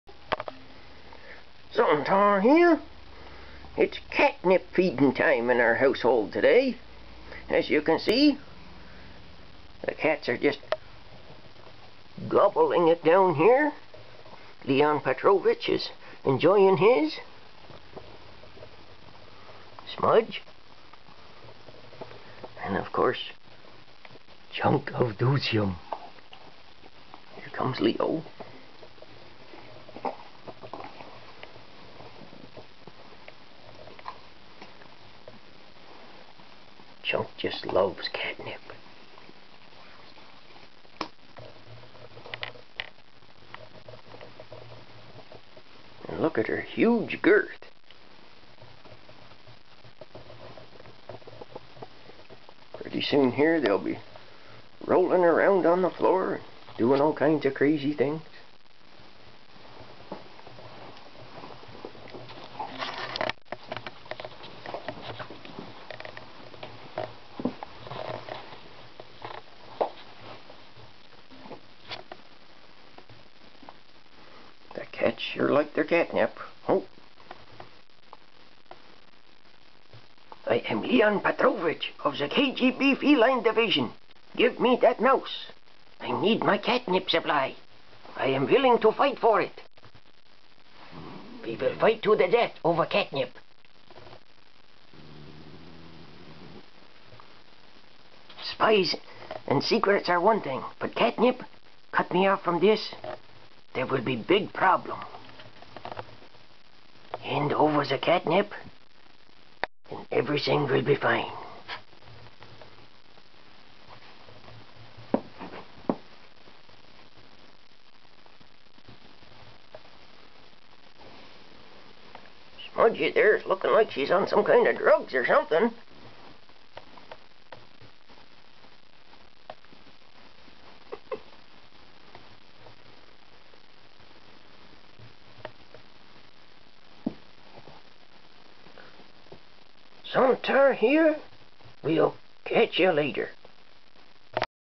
Zontaar's 3 Cats Luv Cat Nip!!!
Chunk, Smudge and Leon Petrovich go Bananas over cat nip, they just luv the kitty drugs!
The 3 cats work harder on the Deltic when they are happy kitties!